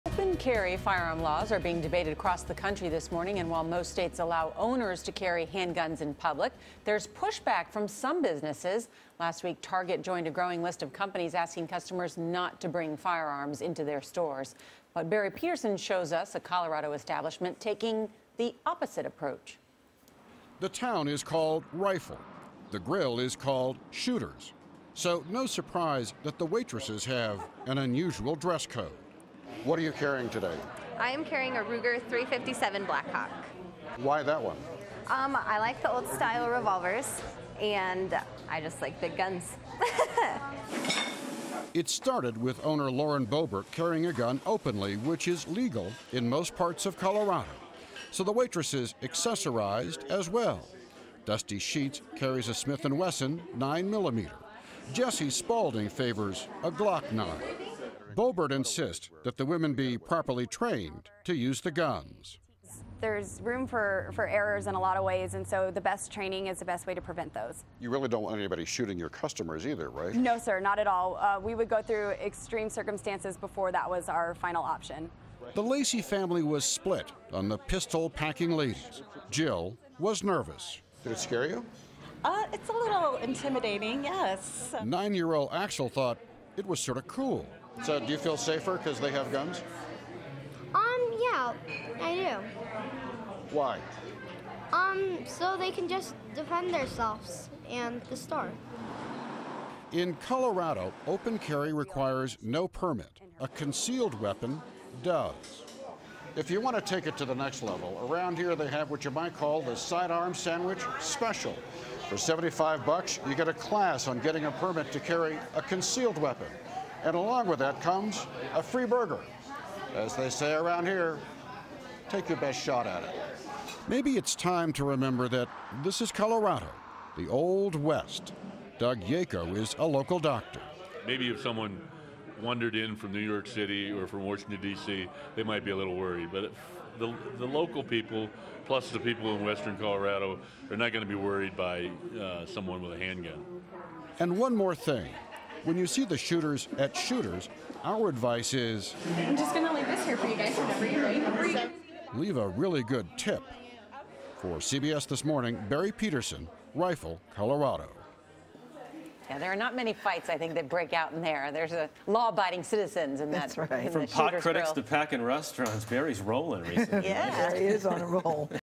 Waitresses openly carry guns at restaurant in Rifle, Colorado
While "open carry" firearm laws are being debated across the country, there's been a push back from some businesses about allowing owners to carry handguns in public. Barry Petersen reports on a Colorado establishment taking the opposite approach.